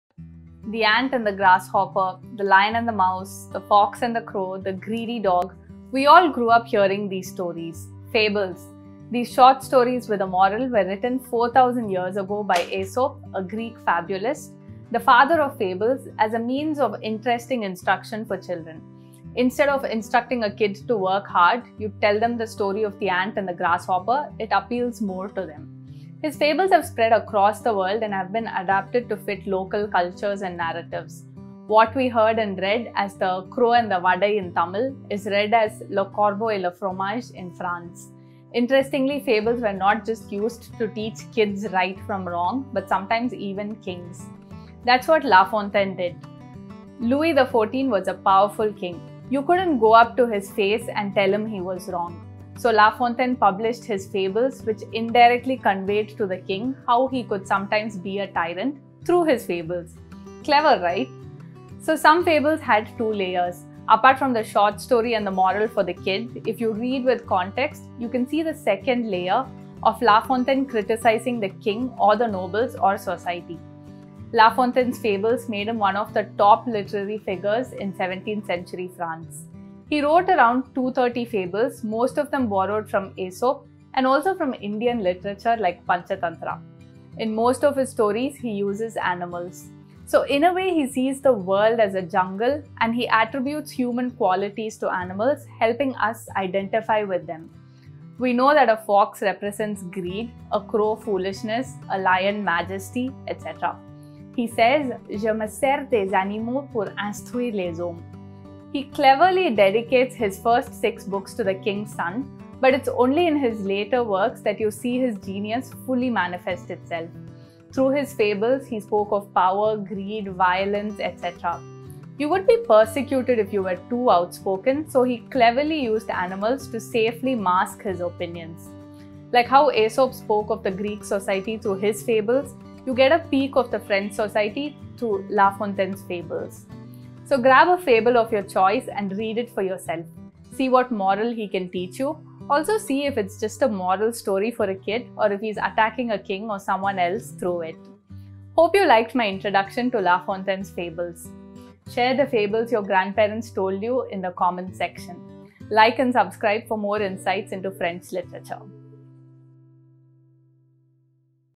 Introduction to la Fontaine's fables
Grandma's tales of the hare and the tortoise, the two goats.. we all grew up with them. Find out more about their origins adaptations, especially by the French author la Fontaine.